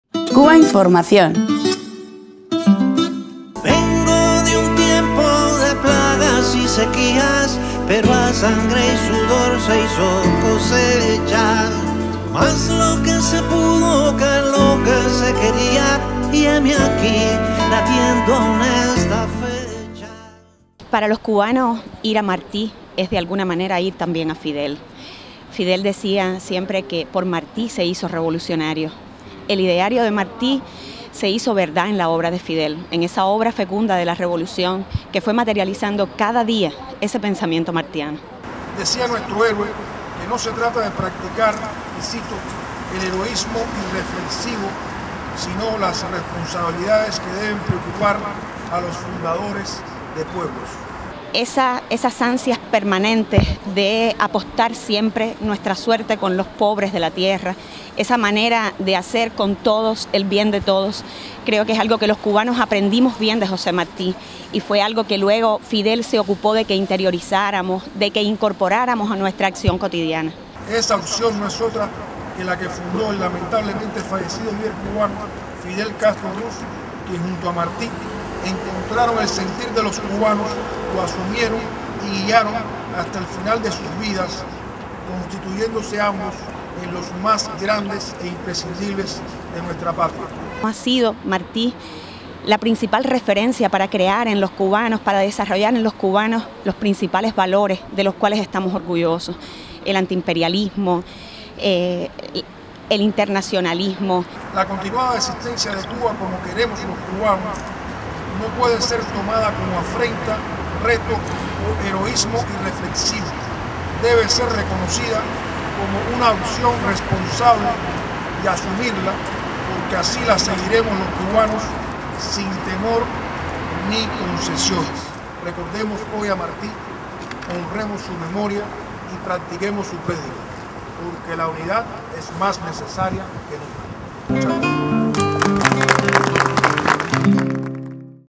Martí y Fidel enseñaron al pueblo cubano a apostar su suerte con pobres de la Tierra
En el acto por el Natalicio 164 del prócer de la independencia cubana José Martí, celebrado en Madrid el 27 de enero de 2017, Cubainformación TV entrevistó a Edelys Santana, Primera Secretaria de la Embajada y especialista en la obra de Martí, quien afirmó que `José Martí y Fidel Castro enseñaron al pueblo cubano a apostar su suerte con los pobres de la Tierra´.